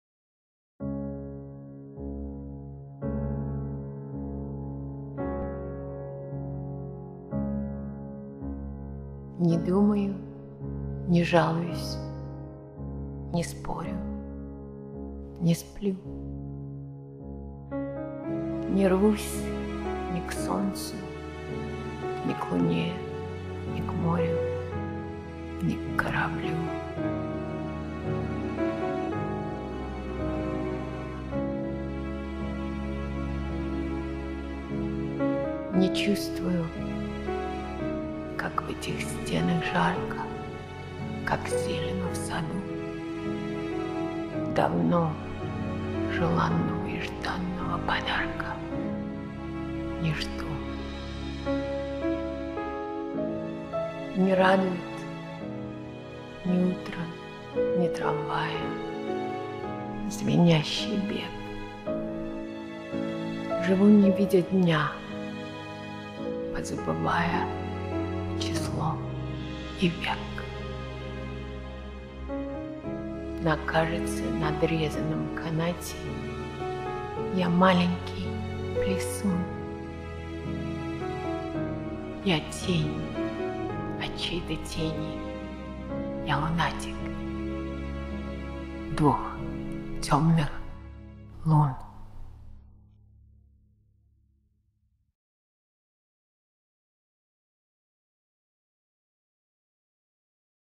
М.Цветаева «Не думаю, не жалуюсь, не спорю…» /M. Tzvetayeva
Images: Mila Gee

"I do not think, or argue, or complain.
Or sleep.
I long for neither sun, nor moon, nor sea.
Nor ship.

I do not feel the heat amidst these walls,
Nor garden’s green,
Nor do I long for your desired gift,
Foreseen.

Neither the morning gladdens nor the trolley’s
Ring-singing run.
I live, forgetting date and age
And daylight sun.

I am – a dancer on a tightrope slashed
And hewn.
I am – a shadow’s shadow: lunatic
Of two dark moons. "
1914
Translated from the Russian by Ekaterina Rogalsky